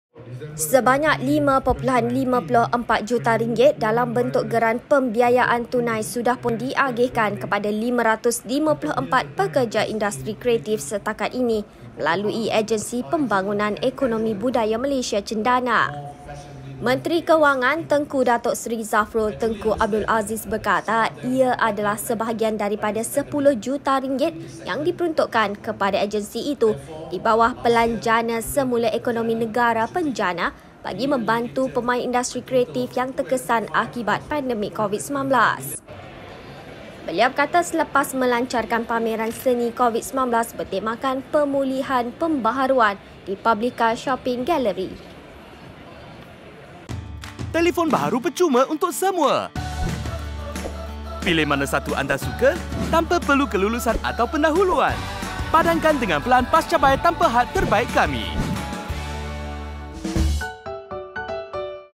RM5.54 juta diagih kepada warga industri kreatif melalui CENDANA
Sebanyak RM5.54 juta dalam bentuk geran pembiayaan tunai sudah pun diagihkan kepada 554 pekerja industri kreatif setakat ini melalui Agensi Pembangunan Ekonomi Budaya Malaysia (CENDANA).

Menteri Kewangan, Tengku Datuk Seri Zafrul Tengku Abdul Aziz, berkata ia adalah sebahagian daripada RM10 juta yang diperuntukkan kepada agensi itu di bawah Pelan Jana Semula Ekonomi Negara (PENJANA) bagi membantu pemain industri kreatif yang terkesan akibat pandemik COVID-19.

Beliau berkata selepas melancarkan Pameran Seni COVID-19 bertemakan "Pemulihan: Pembaharuan" di Publika Shopping Gallery, hari ini.